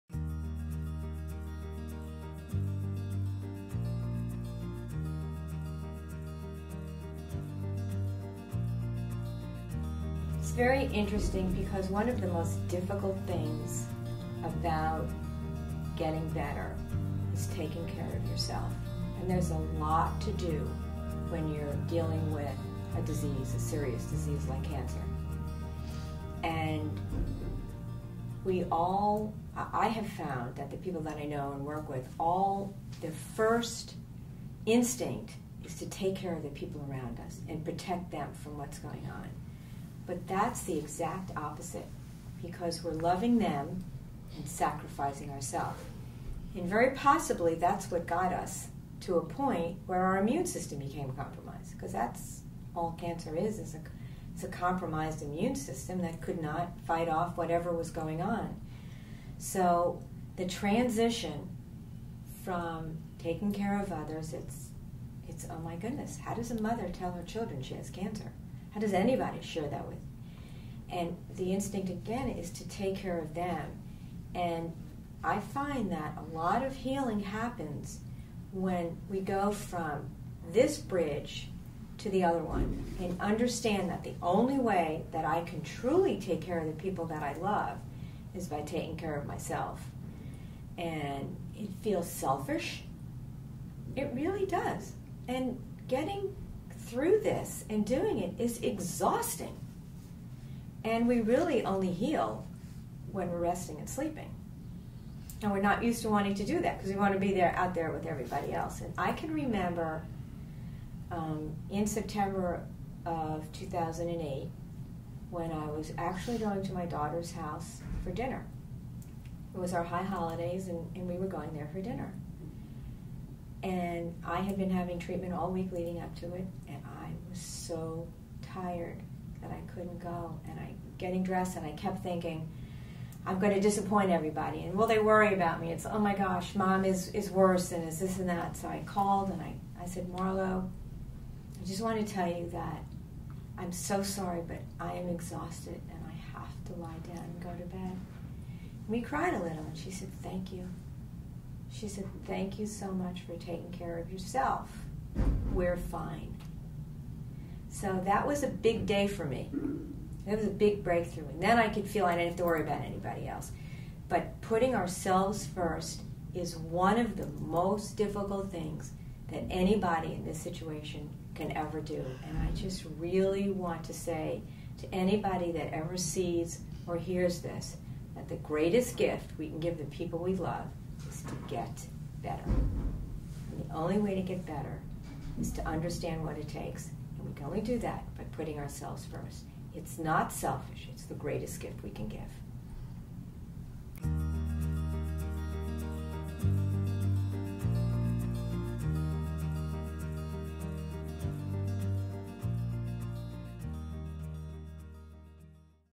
Elaine Gibson Reversed Stage 4 Cancer - Taking Care of Yourself First
Elaine Gibson is an extraordinary woman.

In 2008, she successfully beat Stage IV cancer without traditional protocols, and since then has gone on to share her secrets for recovery, rejuvenation and reinvention with thousands of men and women all around the world. Cited as one of the world’s top ten natural cancer survivors by Extreme Health Radio (ranked #4) she is a walking example of positivity, radiance and healing who has turned her near-tragedy into triumph for the benefit of mankind.

During her journey back to health Elaine lost 28 pounds and dropped 5 dress sizes. Not only that but she came out the other side looking and feeling better than she did before her diagnosis!